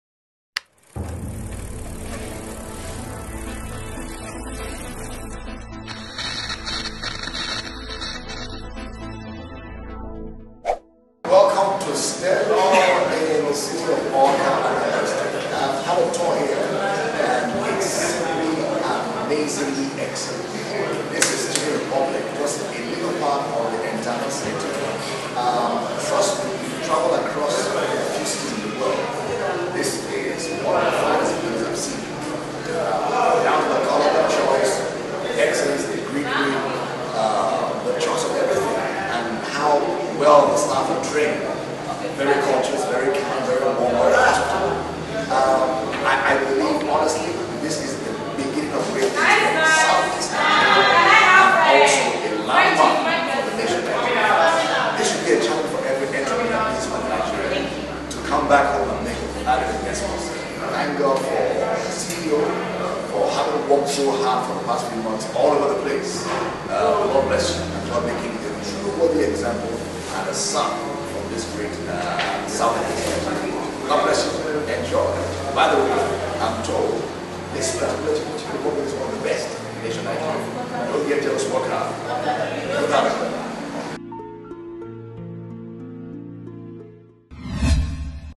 Stanel Awka is One
We’ve had a tremendous 1 year run! July 8 is close! Looking back to the commissioning of the Stanel Ultra modern facility in Anambra State, it’s not out of place to say that indeed Stanel World has changed the face of AWKA forever!

The “One stop facility” as described by many because of its many incredible features was commissioned in 2017 and graced with the presence of the Executive Governor of Anambra State. Through this facility, many Jobs have been created for youths in Anambra and many believe that Anambra would be perceived positively with many more people wanting to visit.

Also in attendance was our ambassador Pastor Goodheart O. Ekwueme who was excited about the facility. He describes the facility as the finest establishment he has come across. He goes on to explain that the facility exudes such excellence that it is a must visit for anyone coming to the eastern part of Nigeria.

Stanel continues to bring excellence to communities as well as put them on the map. Stanel group is really transforming the Nigerian Horizon!